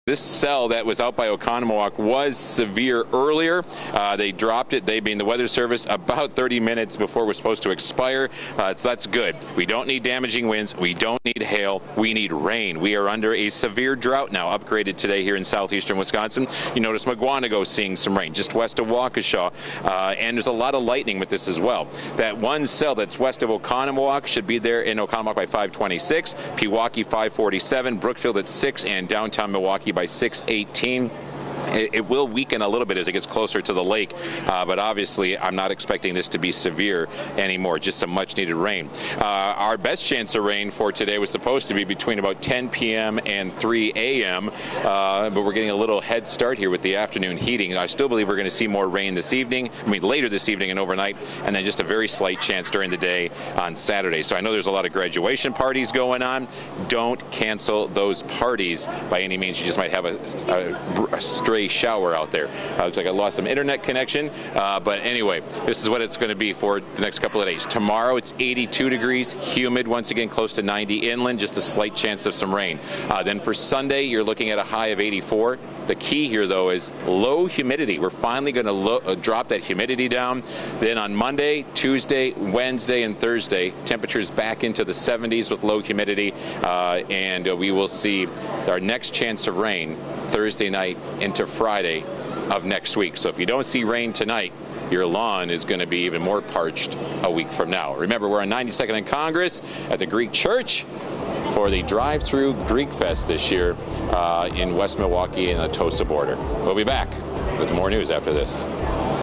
Chance for showers and thunderstorms tonight
Clouds will develop throughout the afternoon, with chance for a few showers popping up as well.  We'll have a better chance for showers and possibly a thunderstorm overnight, especially around midnight.  We'll keep a slight chance for showers into Saturday, otherwise skies will be partly sunny with highs near 80° by the lakefront and 85° inland.  Less humid air moves in for Sunday, with sunny skies.  Highs will be near 82° by the lakefront and 87° inland. High pressure builds in for the rest of next week and will keep us dry.